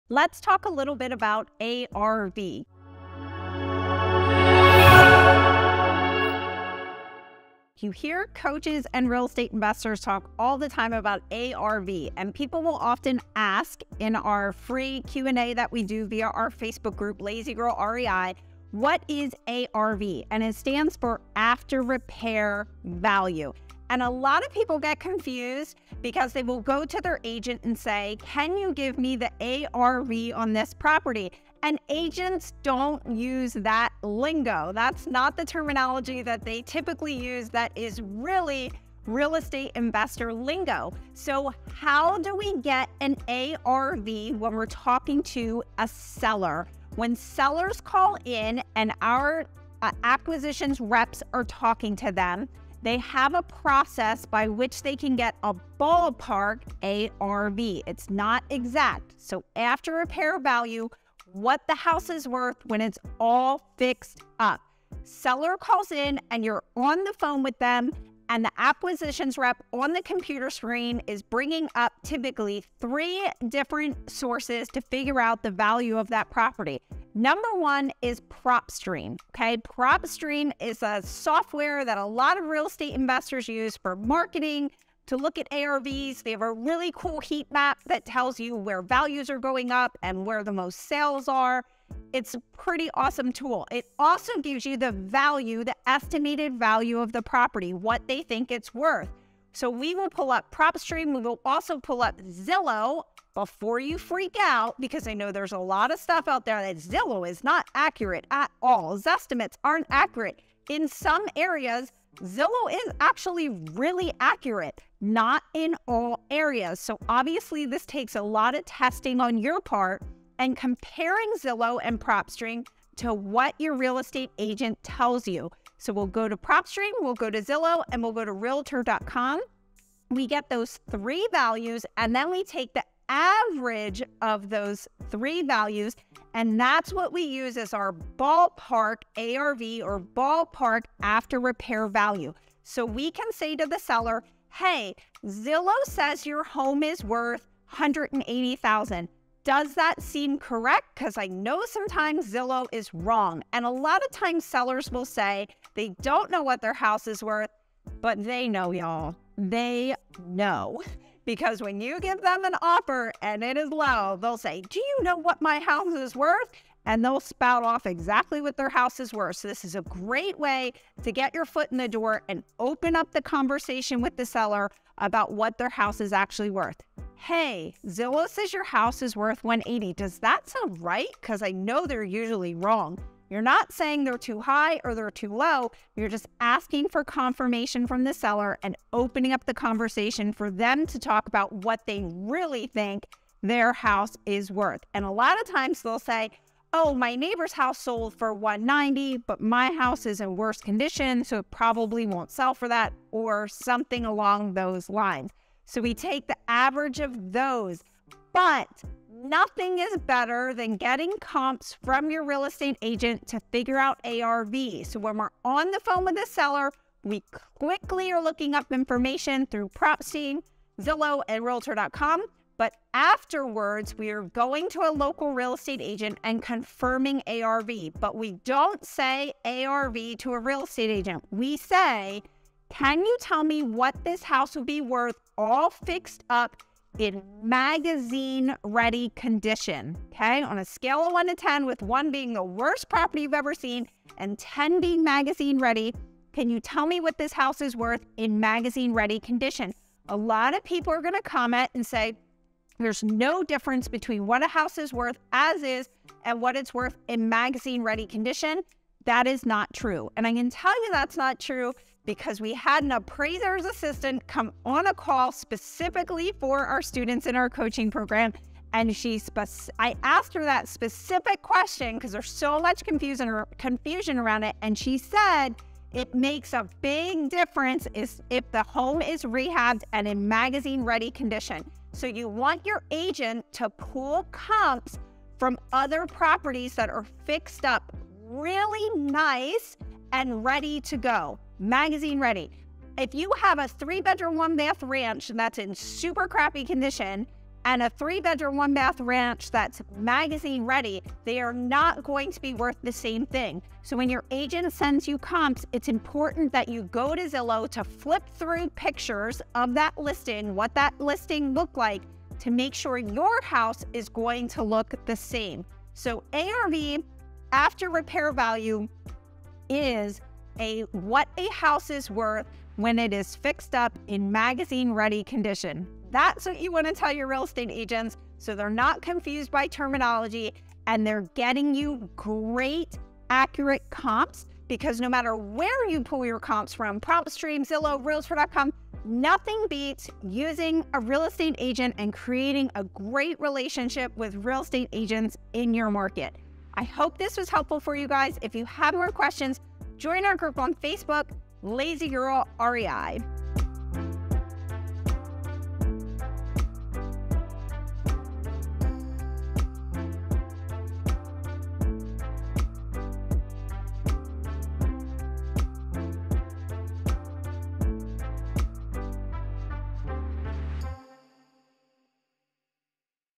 How to Determine The After Repair Value Of A Property
ARV stands for after repair value, a number that’s quite important in every real estate deal you’re going to find. It is one of the numbers that will help you determine if the deal will make sense for you or not, so you need to have a clear understanding of how it works.

Watch this video to learn more about ARV and how you can get this number as accurately as possible!

00:00 Introduction
00:20 What is ARV?
01:17 What resources can you use to determine the ARV?
02:24 How can you use the ARV to negotiate prices?
03:29 Why are comps from real estate agents more reliable?
04:10 What difference does it make if you get the house magazine-ready?
05:15 How can you ask your real estate agent about the ARV?

❓ Ask Me a Question for my Upcoming Youtube Series ❓

🏠 MUST WATCH VIDEOS FOR INVESTORS  🏠